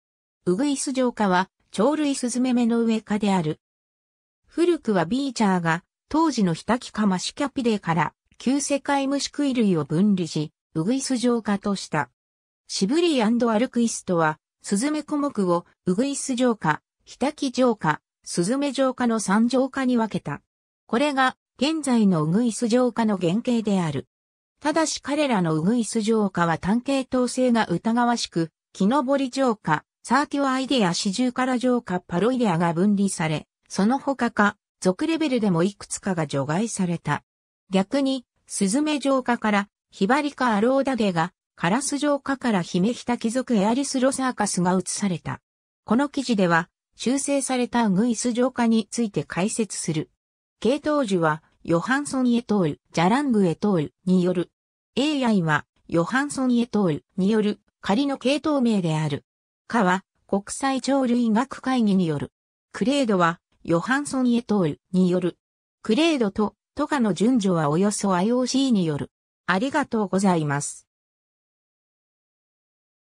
ウグイス上科
ウグイス上科（ウグイスじょうか、学名：Sylvioidea）は、鳥類スズメ目の上科である。
 古くは Beecher (1953) が、当時のヒタキ科 Muscicapidae から旧世界ムシクイ類（ほぼ当時のウグイス亜科、のちのウグイス科に当たる）を分離し、ウグイス上科とした。
 Sibley & Ahlquist (1990) は、スズメ小目をウグイス上科・ヒタキ上科・スズメ上科の3上科に分けた。
これが現在のウグイス上科の原型である。
 ただし彼らのウグイス上科は単系統性が疑わしく、キノボリ上科 Certhioidea・シジュウカラ上科 Paroidea が分離され、そのほか科・属 レベルでもいくつかが除外された。
逆に、スズメ上科からヒバリ科 Alaudidae が、カラス上科からヒメヒタキ属 Erythrocercus が移された。
 この記事では、修正されたウグイス上科について解説する。
 系統樹は Johansson et al. (2008); Gelang et al. (2009)による。
A–I は Johansson et al. による仮の系統名である。
 科は国際鳥類学会議 (IOC)による。
clade は Johansson et al. (2008)による。
clade と科の順序はおよそIOCによる。